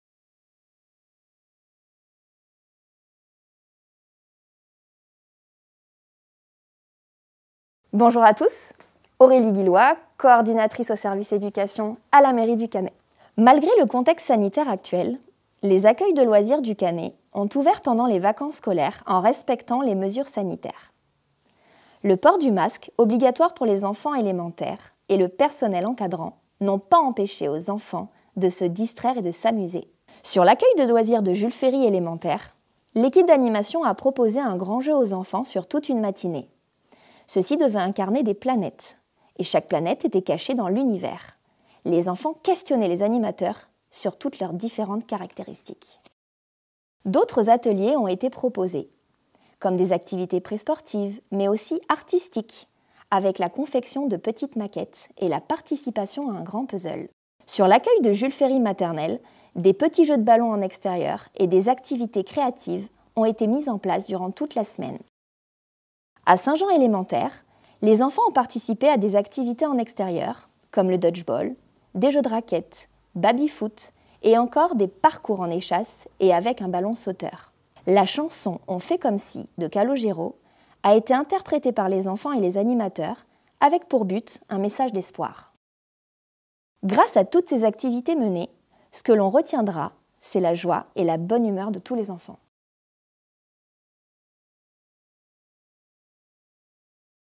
ACCUEILS DE LOISIRS
Les accueils de loisirs maternels et élémentaires accueillent les enfants dont les parents entrent dans la liste des professions prioritaires, définie par l’Etat.
De nombreuses activités leur sont proposées.
➡️   Renseignements auprès du service de l’Éducation :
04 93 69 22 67